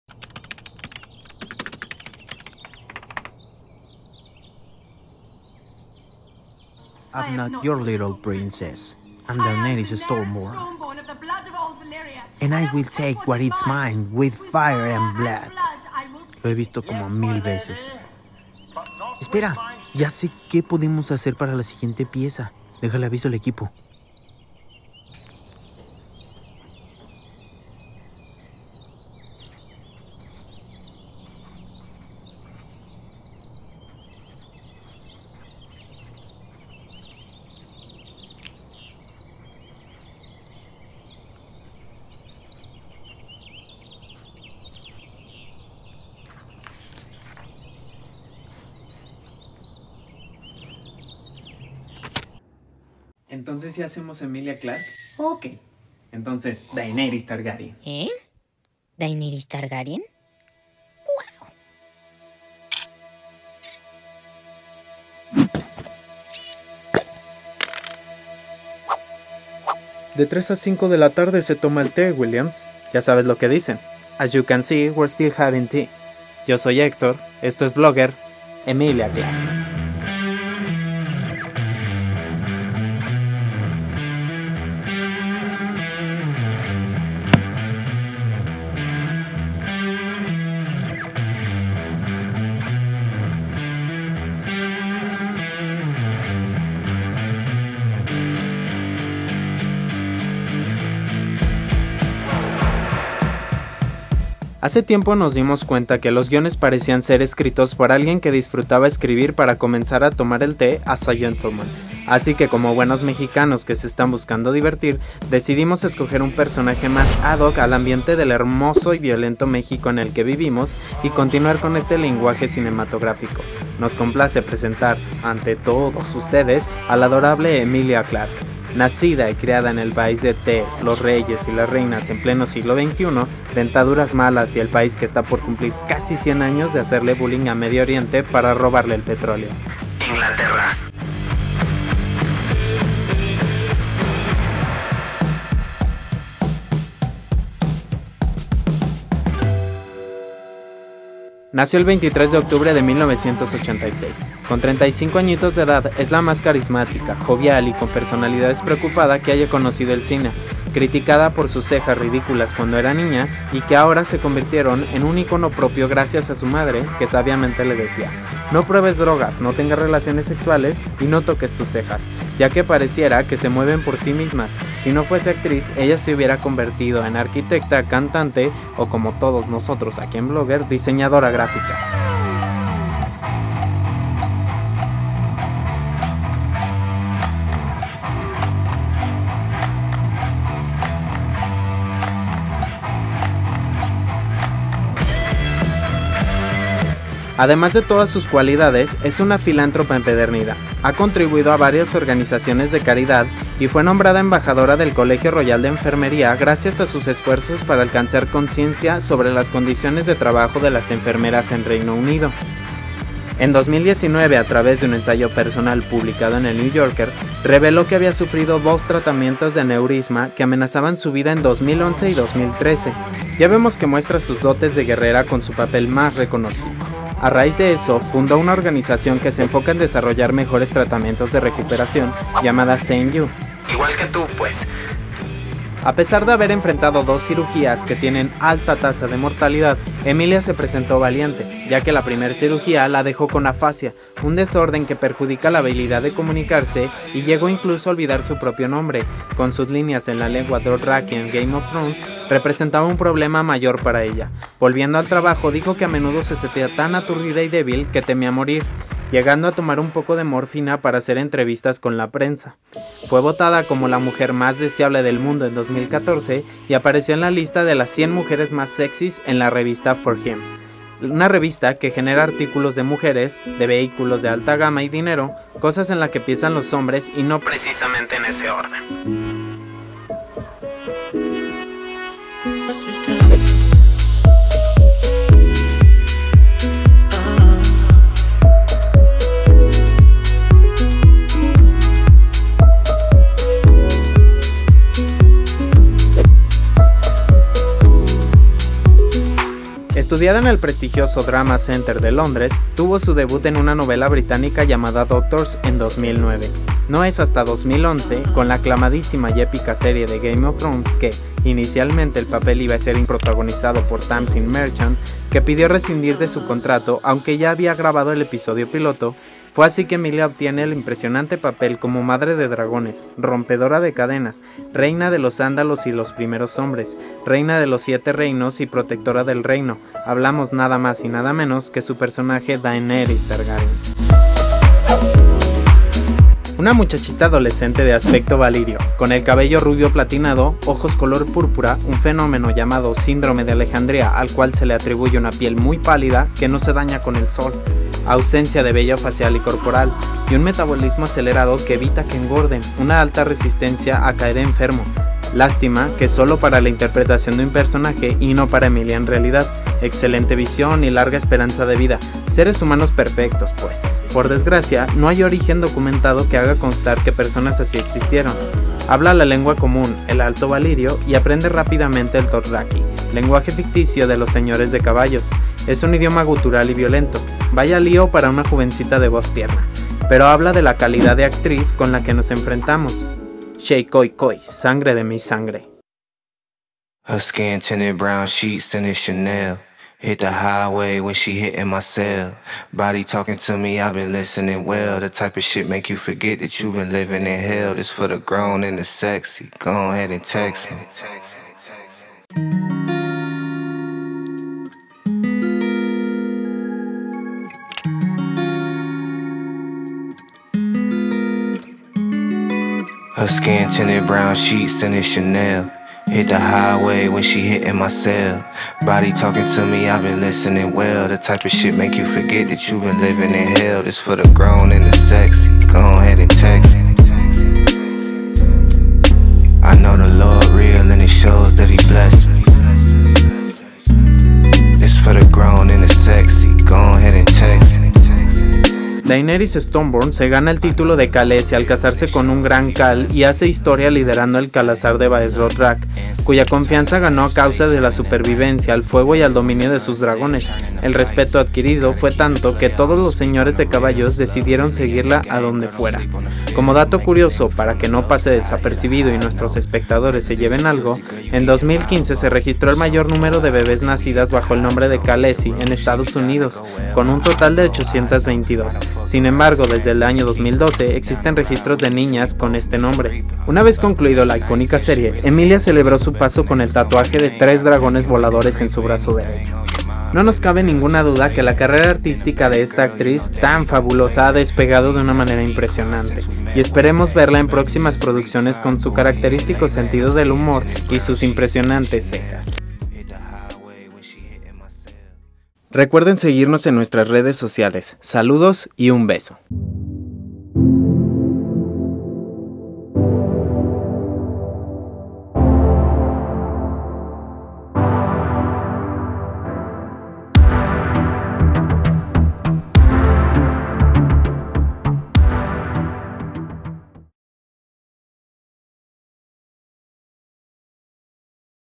Aneurisma Cerebral de EMILIA CLARKE y cómo salir adelante. Vida y trabajo de la Madre de dragones.
Les súper presentamos a Emilia Clarke, ídola para este canal, diosa para la fantasía literaria y el cine moderno.